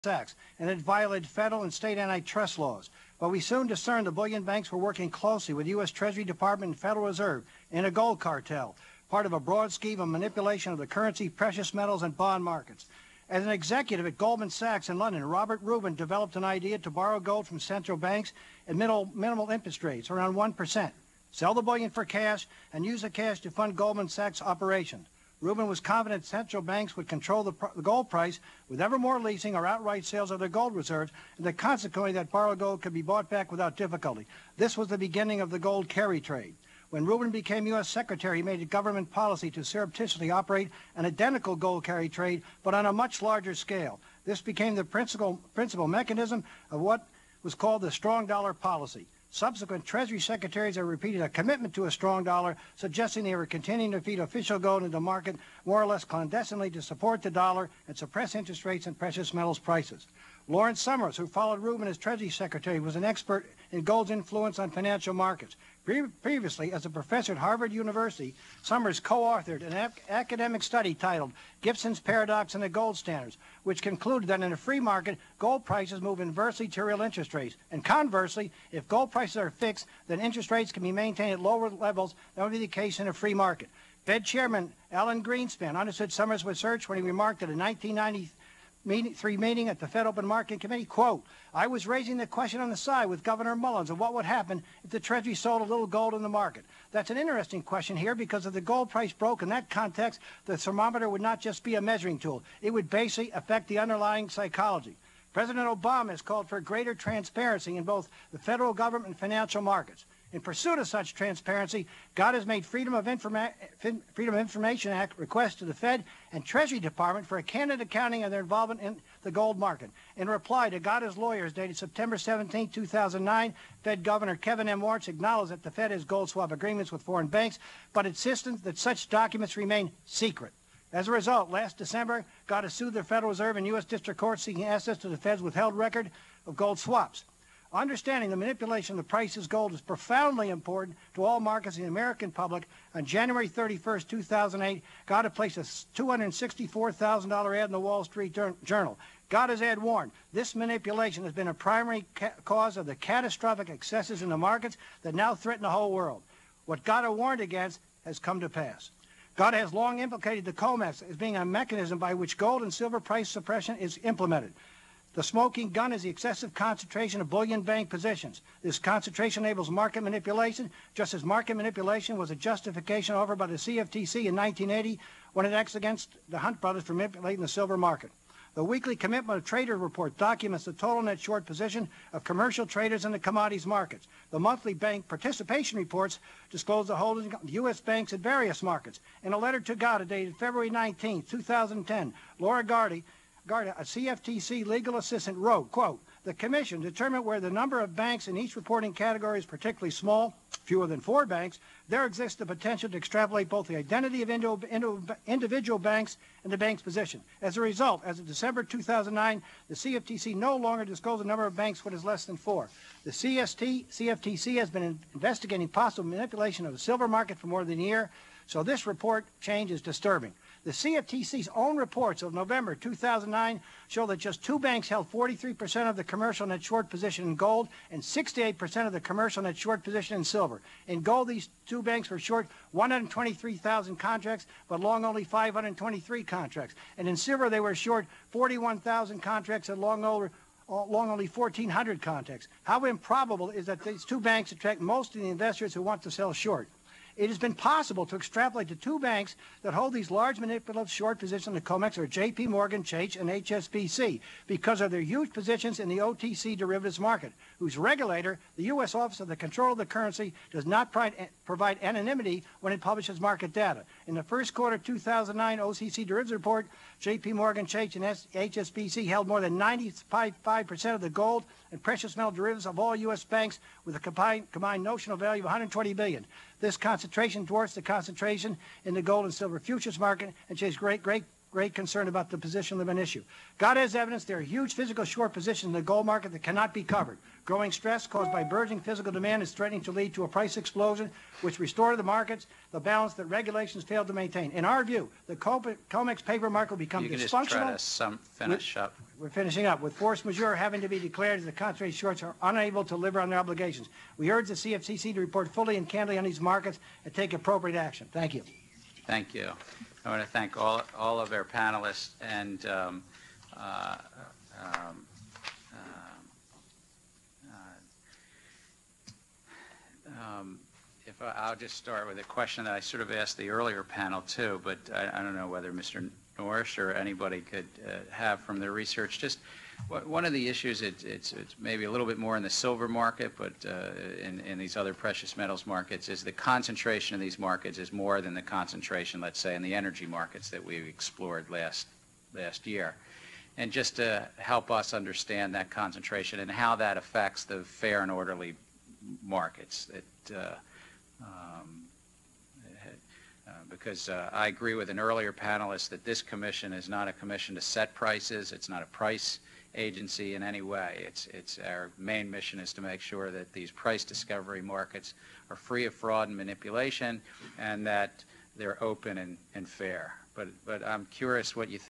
Bill Murphy of GATA Speaks to CFTC
Bill Murphy, Chairman of the Gold Anti-Trust Action Committee delivers his testimony about gold price suppression to the Commodity Futures Trading Commission on 3/25/10.